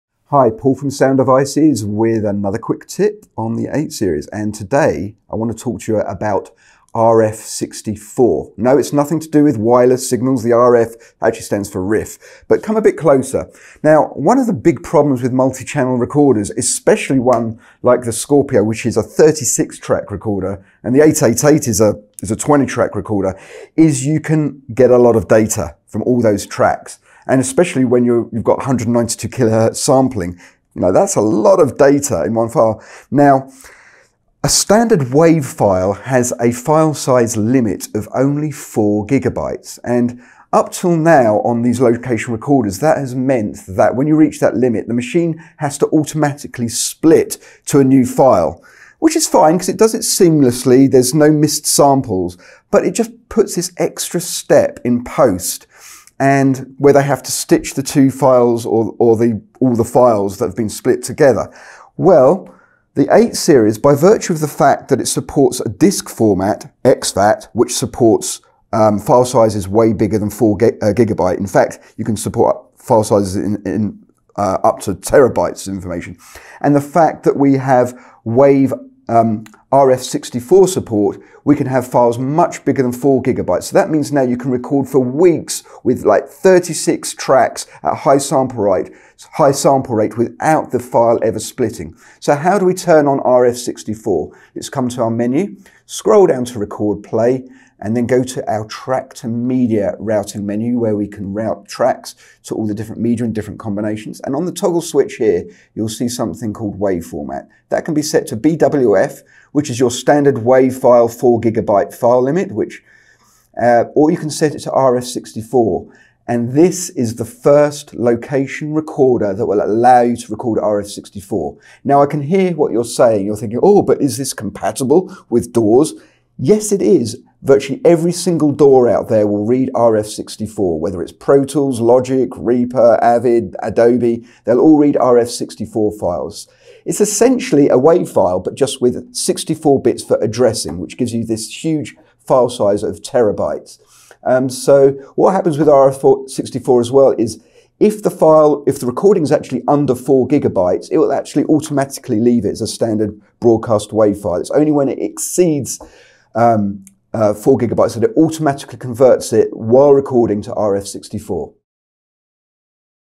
8-Series Quicktip: Say Goodbye to 4GB File Size Limits with RF64
Join Paul as he discusses powerful file recording format on the 8-Series making file splits a thing of the past.